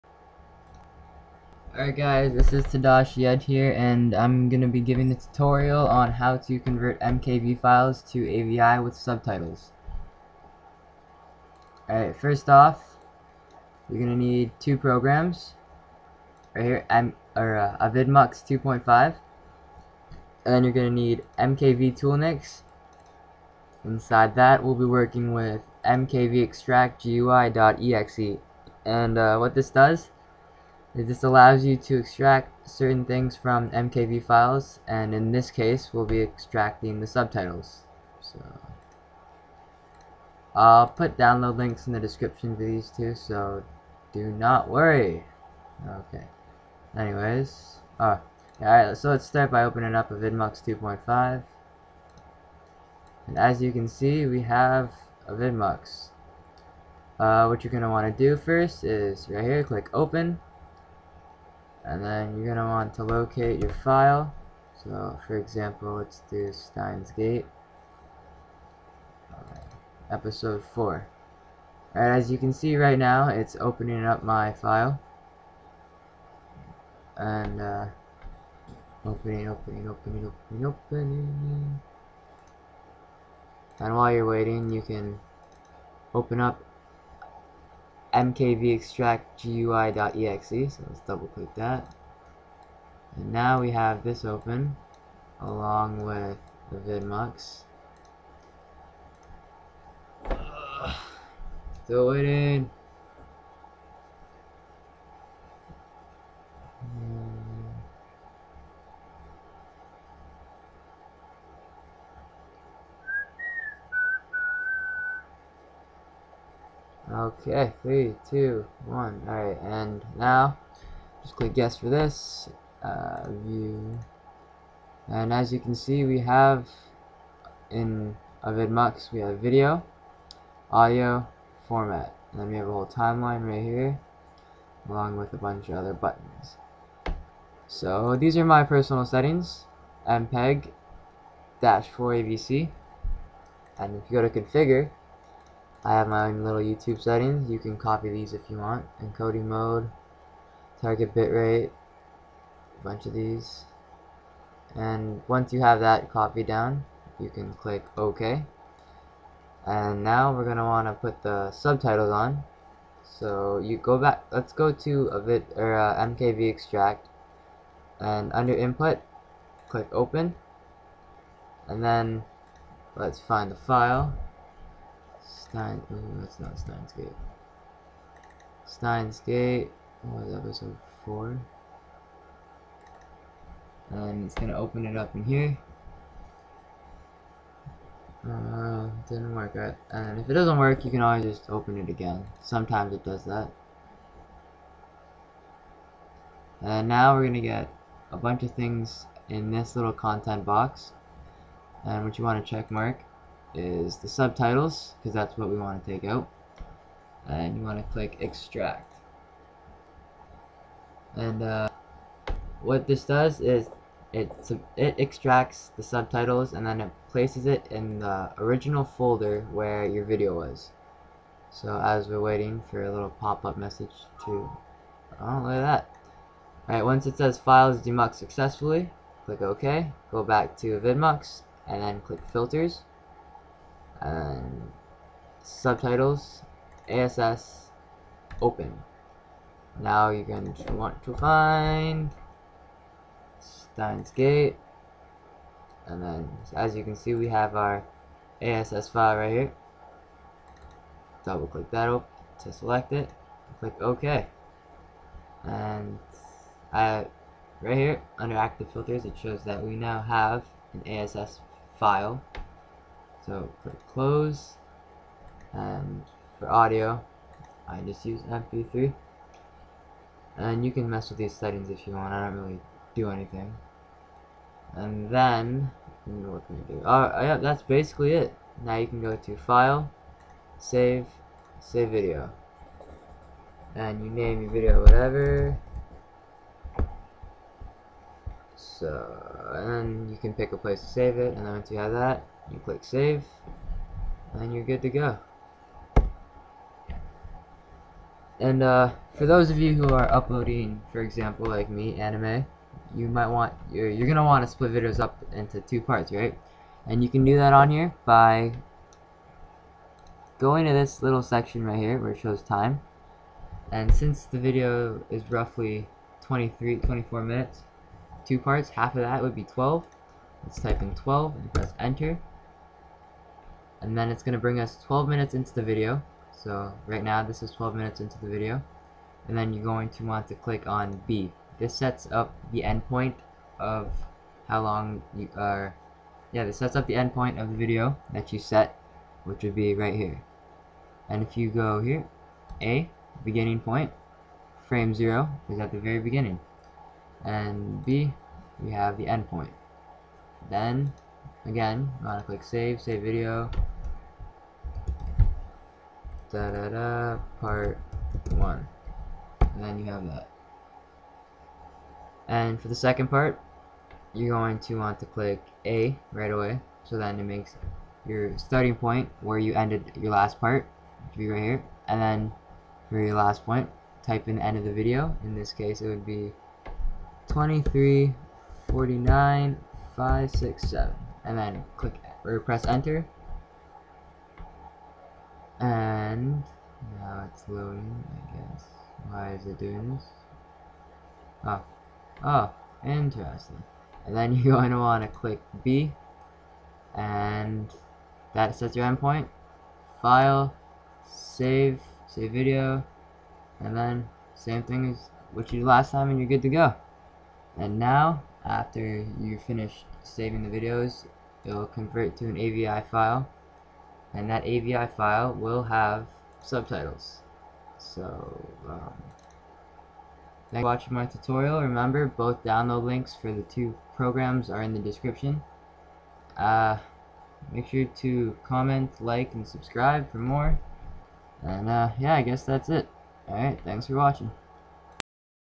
How To Convert mkv. files to avi. WITH Subtitles
Converting MKV. files to AVI.
UPDATE 31/03/2016: Links are dead. This is still super old LOL but I'm sure you can find them if you just Google them. It shouldn't be too different in the newer versions.